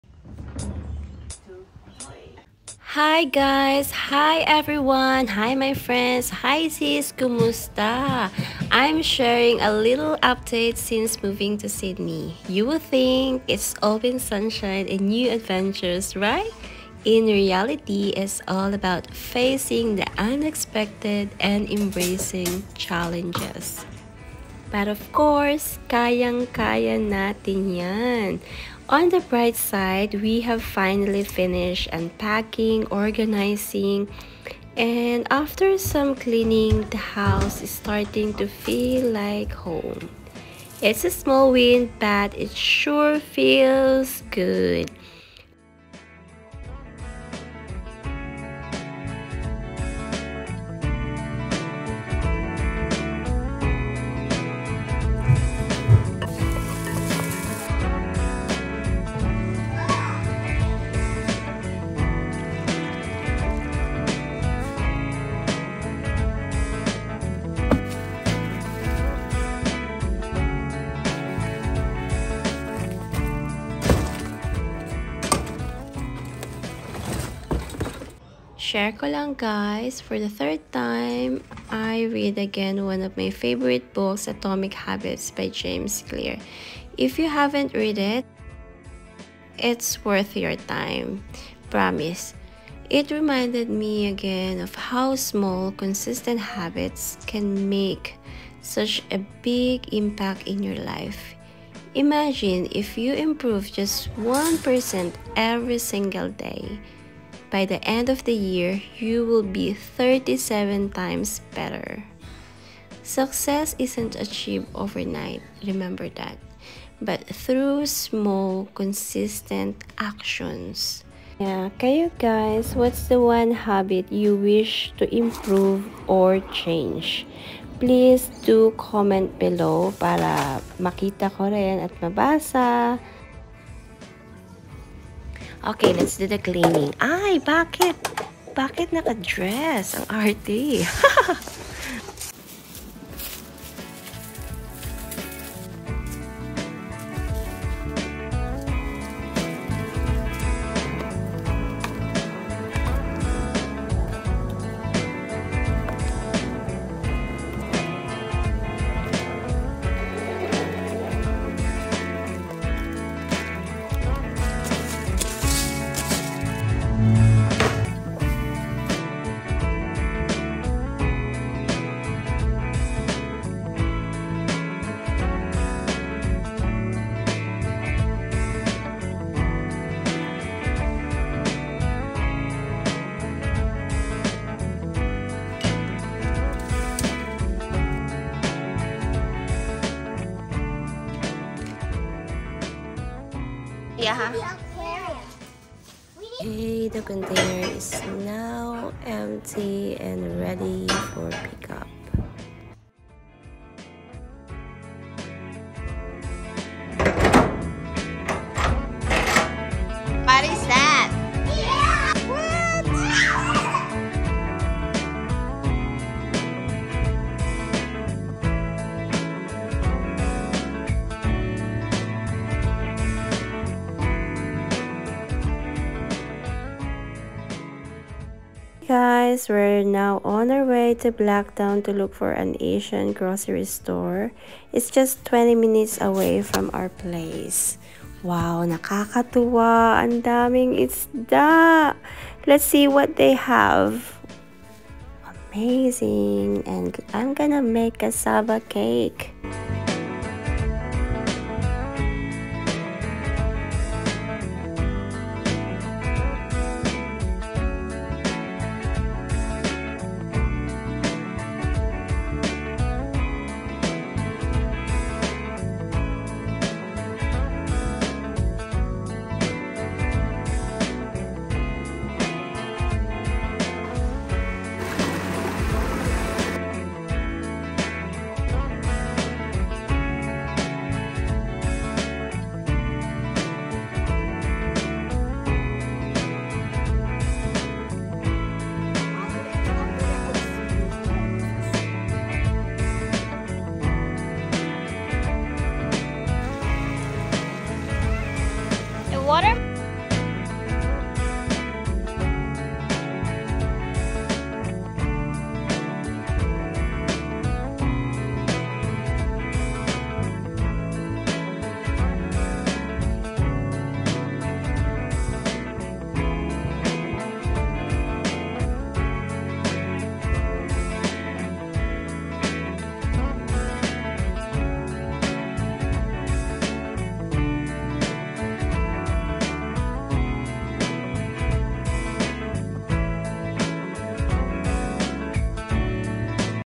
🤩Every Small Action Counts: PROGRESS UPDATE
In this video, I take you through the process of setting up and cleaning our house to transform it into a home. I'll also share my thoughts on 'Atomic Habits', a book I'm currently rereading, a game-changer for me. Learn how tiny changes can make a big impact over time.

If you haven't watched my previous vlog, please check it out! ❤️